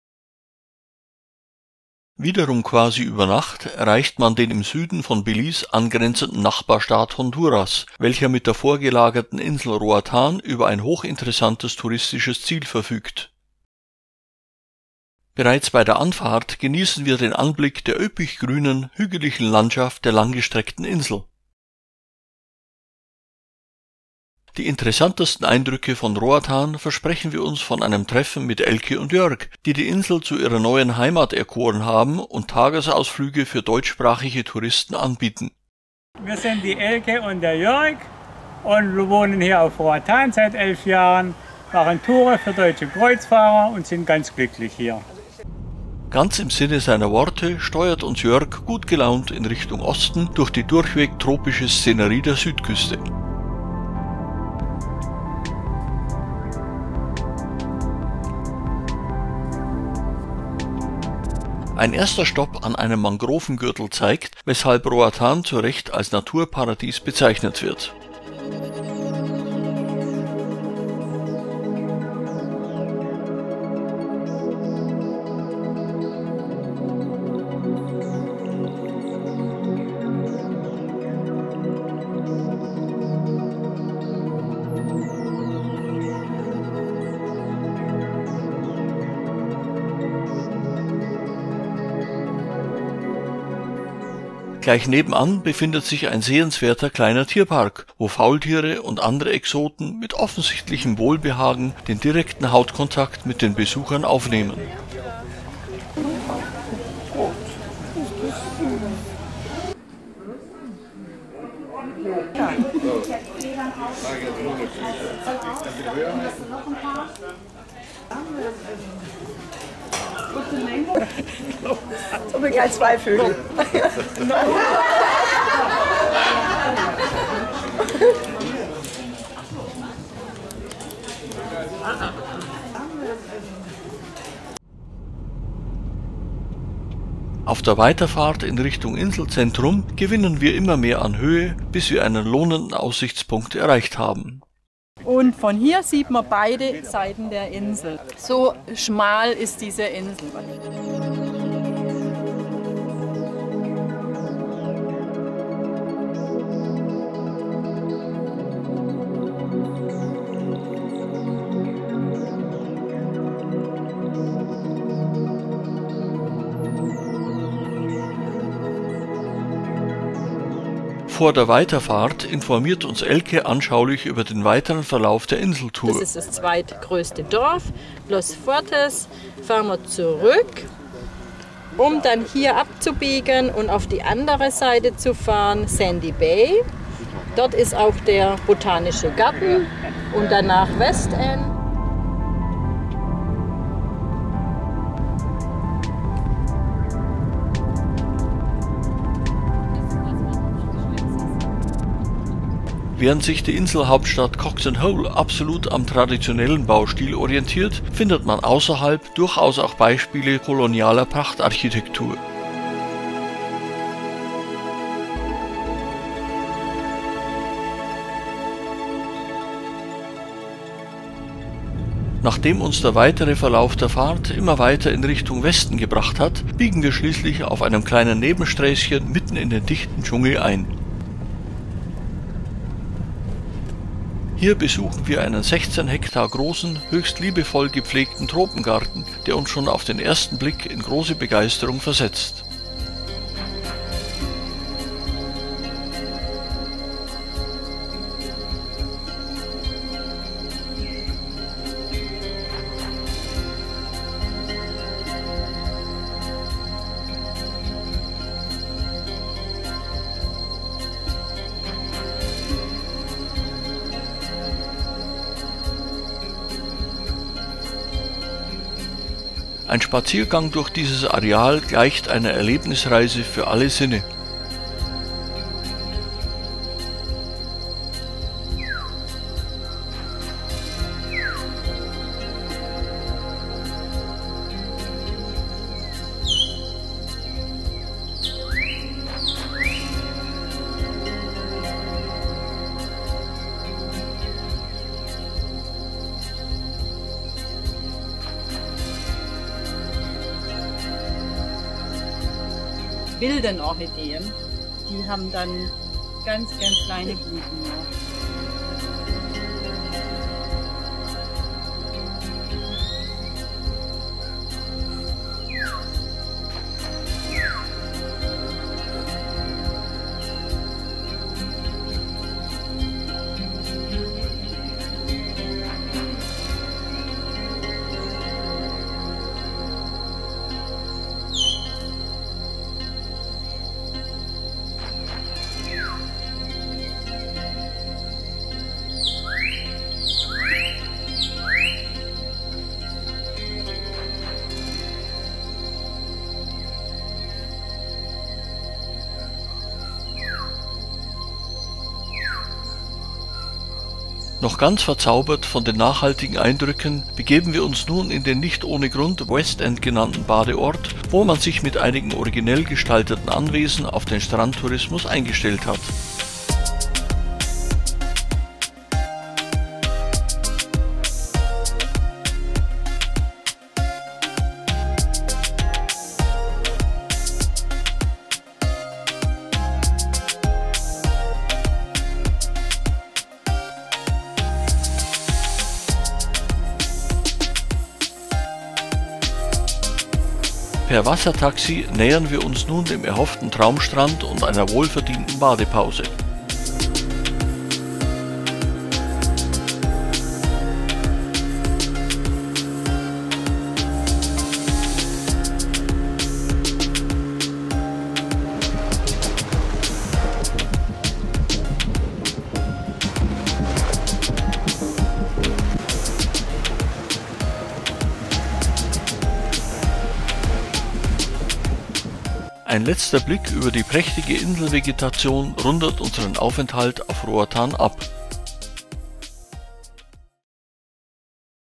Roatan (Honduras): Inselrundfahrt
Die deutschen Auswanderer Elke und Jörg begleiten uns auf einer Inselrundfahrt, die uns wundervolle Tier- und Naturbeobachtungen, markante Aussichtspunkte, unglaubliche botanische Vielfalt und schließlich einen Traumstrand beschert.